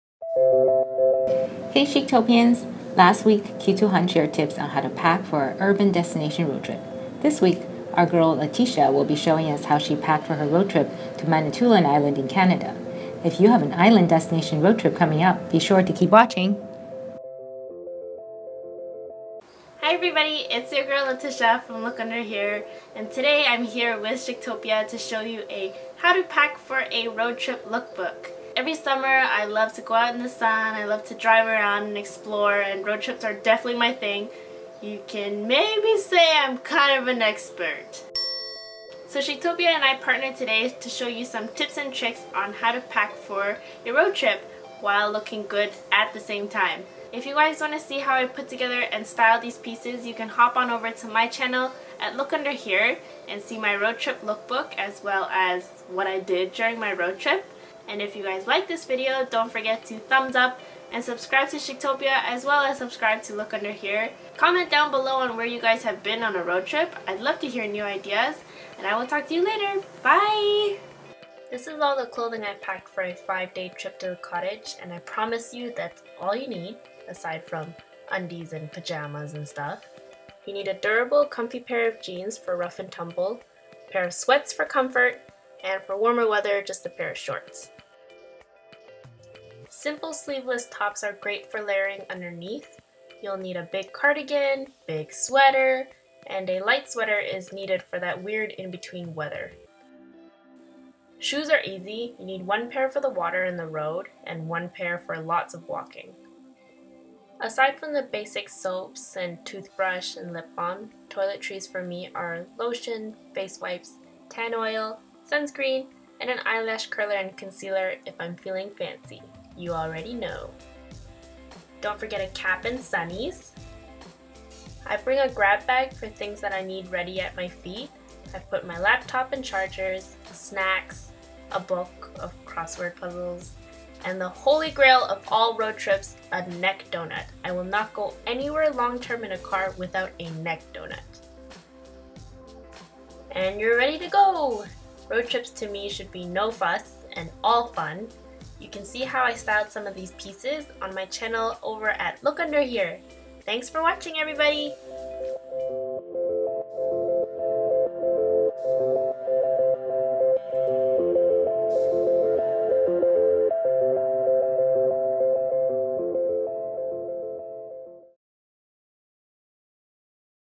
How to Pack for a Road Trip to an Island Destination - Look Under Here x Chictopia
As fashion lovers, we all want the ability to pack our entire wardrobe into one suitcase when going on a road trip. Having options is great, but a car full of suitcases and wrinkled clothes is not. We teamed up with our girl Letitia from LookUnderHere to bring you the ultimate guide on how to pack for a road trip, while still looking as stylish as ever.

Featured Items:
-Jeans
Thrifted
-Pants
-Shorts
DIY
-Shirt
-Sweater
Thrifted
-Sweater

Are you a creative youtube blogger or fashion blogger looking to collaborate with Chictopia? We always want to work with more talents like you!